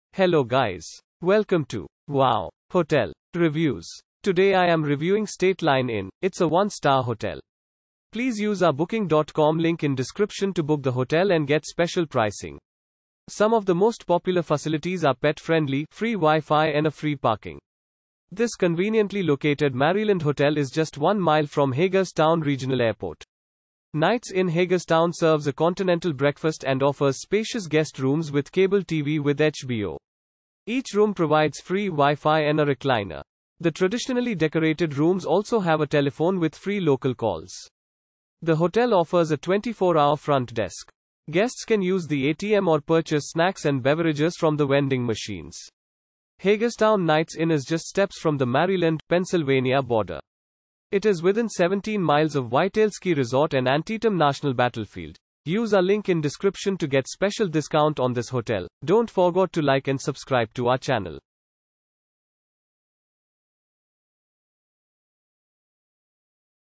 State Line Inn Review - Hagerstown , United States of America
State Line Inn

This conveniently located Maryland hotel is just 1 miles from Hagerstown Regional Airport. Knights Inn Hagerstown serves a continental breakfast and offers spacious guest rooms with cable TV with HBO.
Each room provides free Wi-Fi and a recliner. The traditionally decorated rooms also have a telephone with free local calls.
The hotel offers a 24-hour front desk. Guests can use the ATM or purchase snacks and beverages from the vending machines.
Hagerstown Knights Inn is just steps from the Maryland/Pennsylvania border. It is within 17 miles of Whitetail Ski Resort and Antietam National Battlefield.